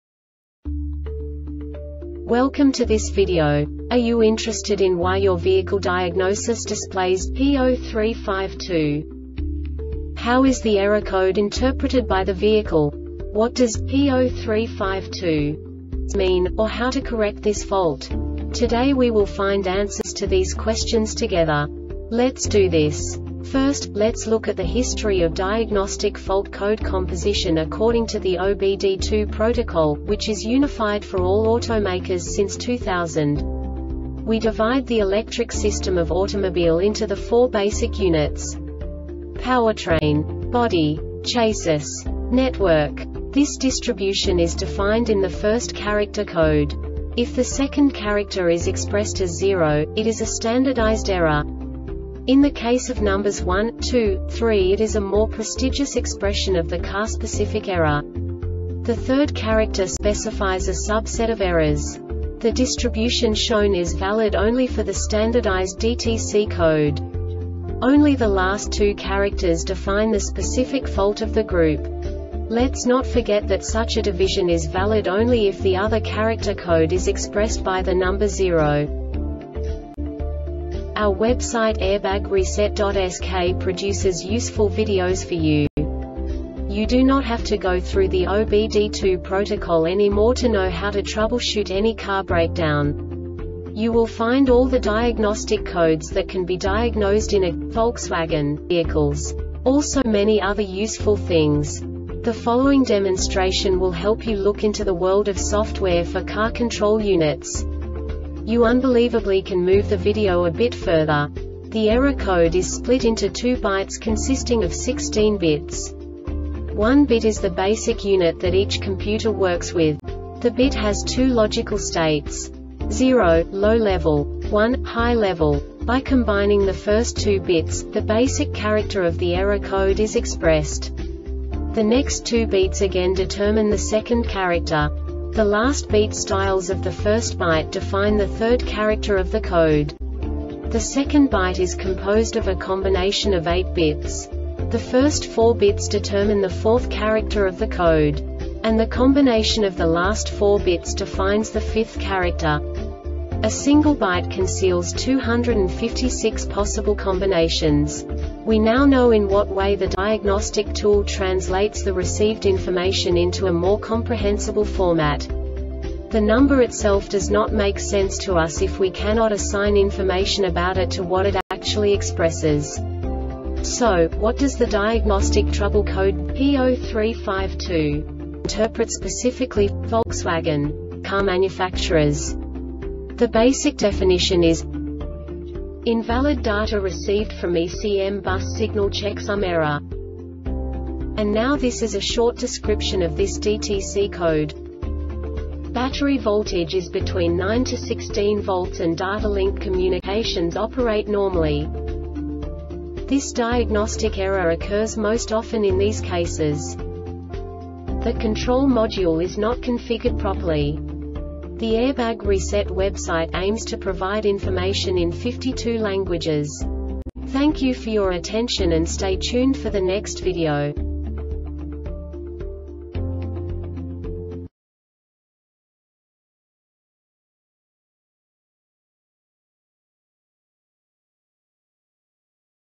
DTC Volkswagen P0352 Short Explanation
Contents:
0:21 Basic DTC analysis according to OBD2 protocol standard.
1:48 Insight into programming
2:58 A diagnostically understandable description of the Diagnostic Trouble Code
3:05 Basic error definition

Make:
Volkswagen
Code:
P0352
Definition:
Invalid Data Received from ECM Bus Signal Checksum Error
Description: